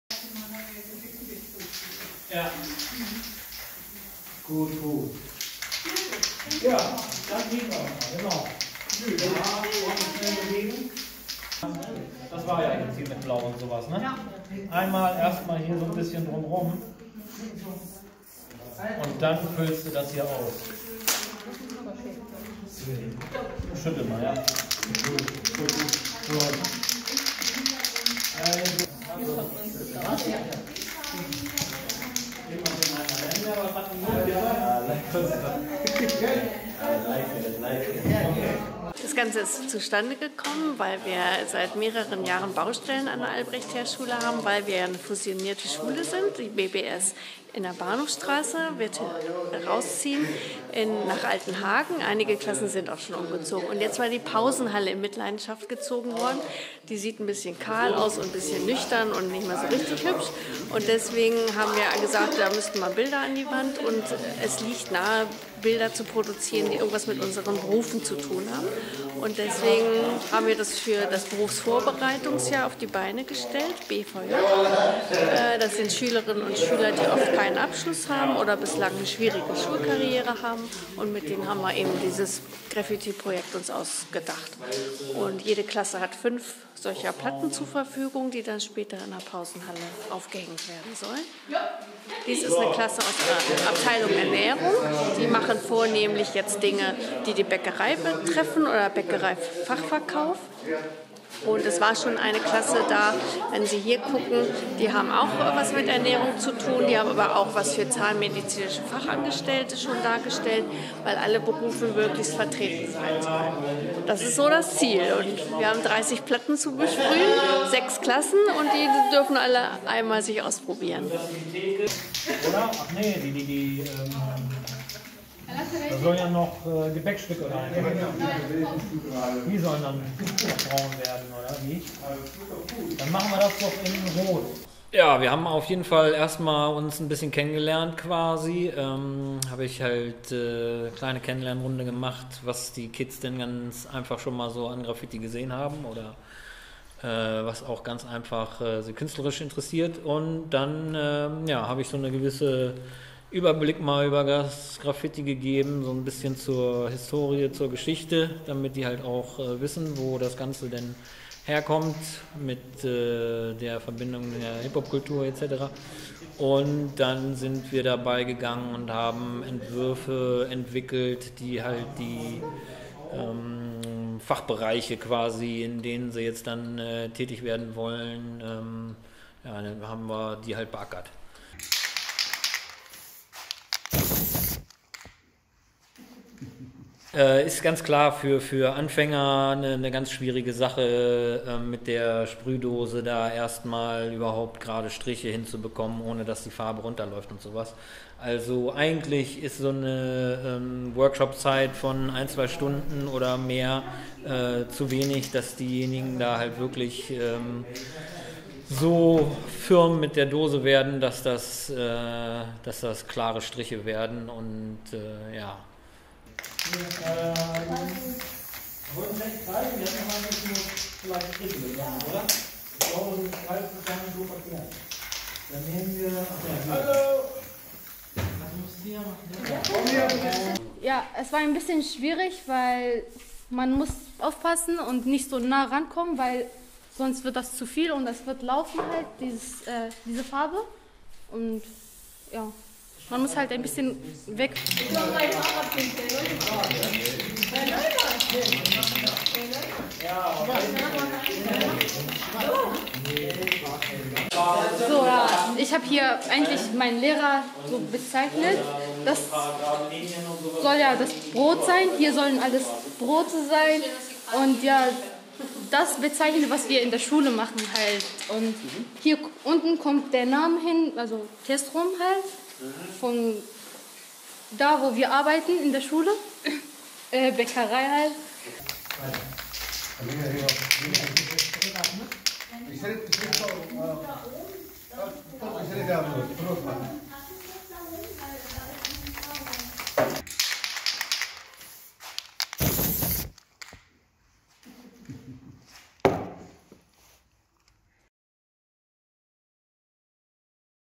Berufsschüler sprayen für die Pausenhalle
An die Dosen, fertig, los! Rund 60 Berufsschüler haben unter Profi-Anleitung Bilder für die noch etwas triste Pausenhalle gestaltet. Die Motive sind ihre Wunsch-Jobs.